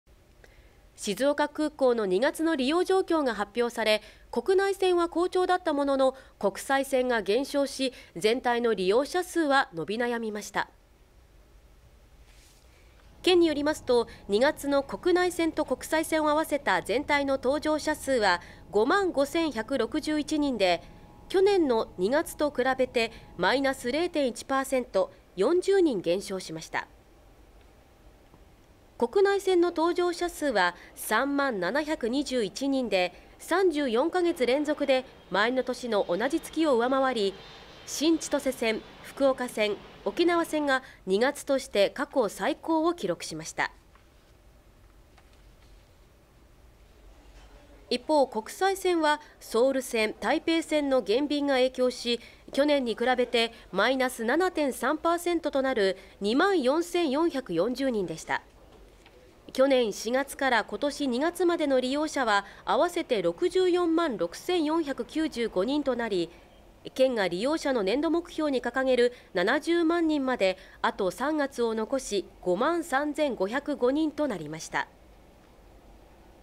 静岡空港 国内線が好調 ２月の利用状況
静岡空港の２月の利用状況が発表され、国内線は好調だったものの国際線が減少し、全体の利用者数は伸び悩みました。
　県によりますと、２月の国内線と国際線をあわせた全体の搭乗者数は５万５１６１人で、２０１８年の２月と比べてマイナス０・１％、４０人減少しました。国内線の搭乗者数は３万７２１人で、３４カ月連続で前の年の同じ月を上回り、新千歳線、福岡線、沖縄線が２月として過去最高を記録しました。一方、国際線はソウル線、台北線の減便が影響し、２０１８年に比べてマイナス７・３％となる２万４４４０人でした。
　２０１８年４月から２０１９年２月までの利用者はあわせて６４万６４９５人となり、県が利用者の年度目標に掲げる「７０万人」まであと３月を残し５万３５０５人となりました。